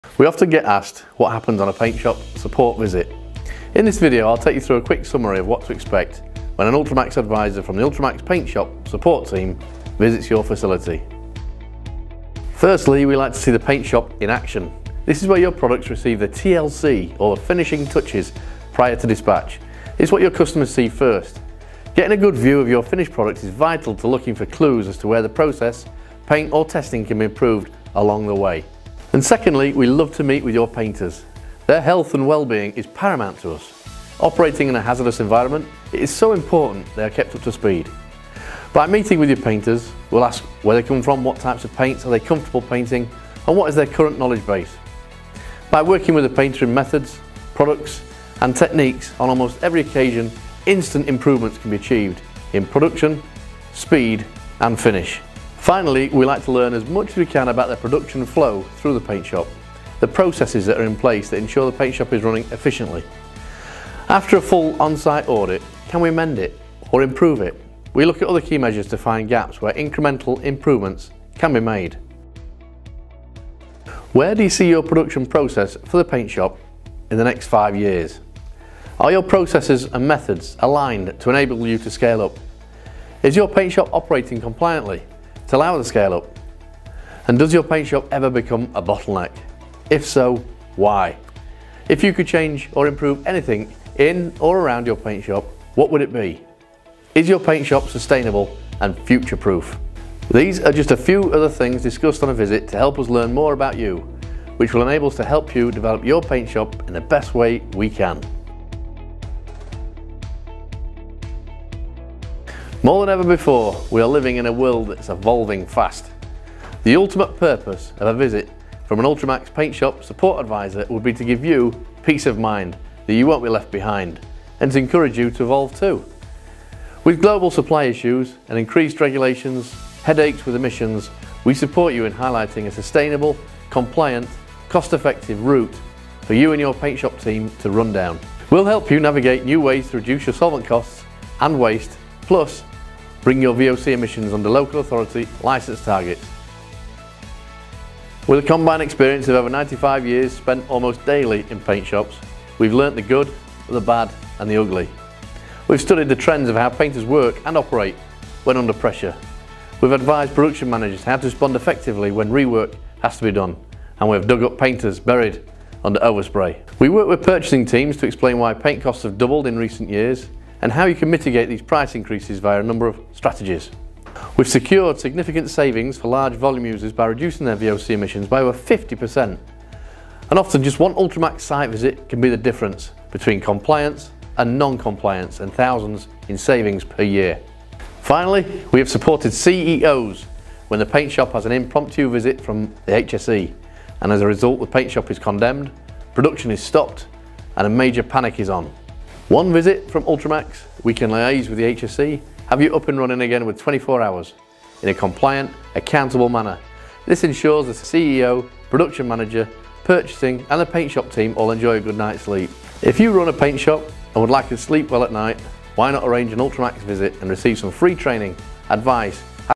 What Happens On An Ultrimax Paint Shop Support Visit?
00:00 - What Happens On An Ultrimax Paint Shop Support Visit?

It may seem like an obvious question but we are quite often asked what does actually happen when Ultrimax personnel turn up for a paint shop visit...some  customers are even unaware we offer a full person to person turnkey service and believe we are only an e-commerce offering!

Well here's a quick video detailing out the full process what we can do to solve your problems and why we are the best !